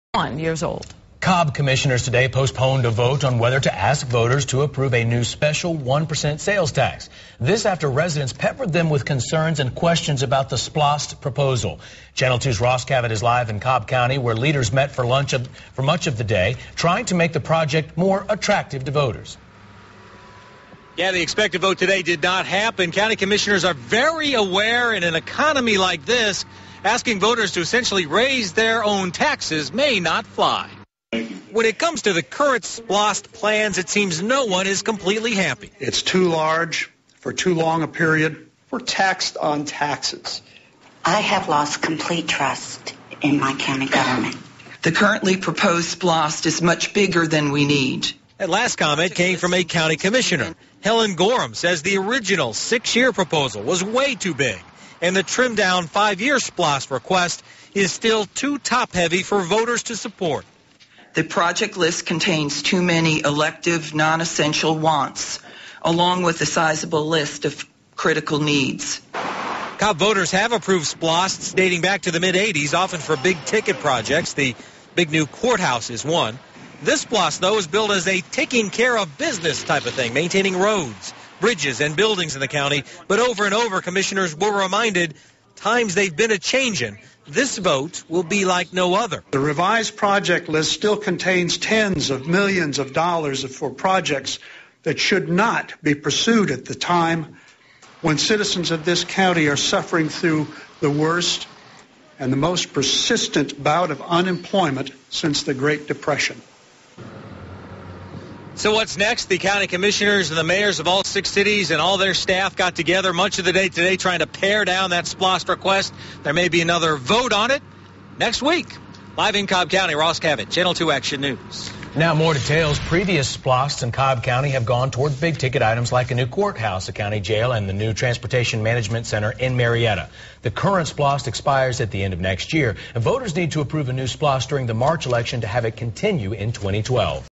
Cobb Commissioners Delay SPLOST Vote
Prior to putting a 4 year $490 million SPLOST on the ballot, the Cobb County Commission met with opposition over their first proposals.  After several delays, the Commission voted 4-1 to place it on the ballot in a special election March 15, 2011.

If you would like to vote yourself a tax cut, please to join the opposition to the proposed SPLOST.